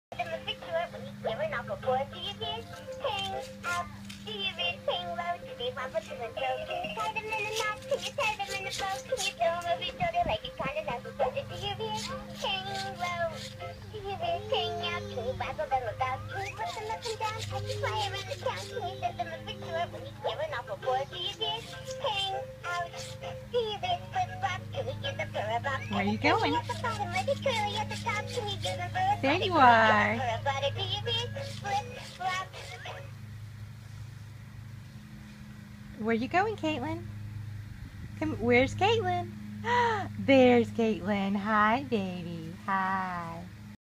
Kaitlyn's workout
Kaitlyn found this dog and song today at wellsfest2013. Now, it's her new workout. Sept 28, 2013, 21 months